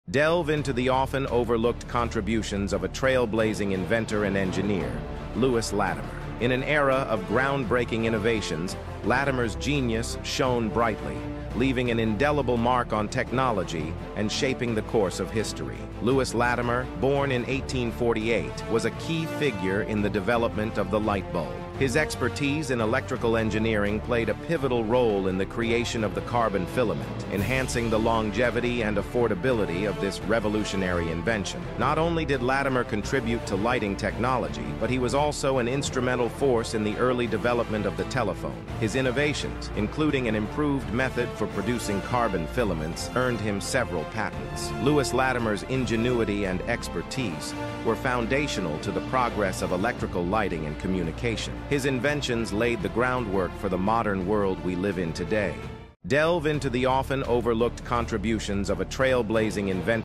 🌟 "Lewis Latimer: Illuminating Innovation | The Untold Story of a Pioneering Inventor" 🌟
🌟 Lewis Latimer: Illuminating the Path to Innovation 🌟

Join us on a journey through the remarkable life and legacy of Lewis Latimer, a trailblazing inventor and engineer whose contributions shaped the landscape of technology in the late 19th and early 20th centuries.

👨‍🔧 Who Was Lewis Latimer?
Lewis Howard Latimer (1848–1928) was a pioneering African American inventor and engineer, born to parents who had escaped slavery. In this video, we delve into Latimer's life story, from his early struggles to his groundbreaking achievements that left an indelible mark on history.

🔧 Key Contributions:
Discover the inventions and innovations that set Lewis Latimer apart. From his crucial work on the carbon filament for the incandescent light bulb to his advancements in electrical engineering, Latimer's brilliance and ingenuity shine through as we explore his major contributions to the field.

👨‍🎓 Educational Advocate:
Explore Latimer's commitment to education and his role as an advocate for African American rights. Learn about his efforts to inspire future generations through his work with the Edison Pioneers and the encouragement he provided to aspiring inventors.

🌐 Latimer's Impact Today:
Uncover the lasting impact of Lewis Latimer's inventions on our modern world. How do his contributions continue to influence technology, and what lessons can we learn from his life and work?

🎥 Archival Footage and Interviews:
Experience rare footage and insightful interviews that provide a more personal understanding of Lewis Latimer's character and the historical context in which he lived.

📚 Recommended Reading:
For those eager to dive deeper into the life of Lewis Latimer, check out the recommended books and articles in the video description. Expand your knowledge and gain a comprehensive understanding of this influential figure.

🤝 Join the Conversation:
Share your thoughts, comments, and questions about Lewis Latimer below. Let's engage in a meaningful conversation about the impact of inventors and pioneers like Latimer on our world today.

Don't miss out on future explorations of influential figures and groundbreaking innovations

Thank you for joining us on this enlightening exploration of the life and legacy of Lewis Latimer.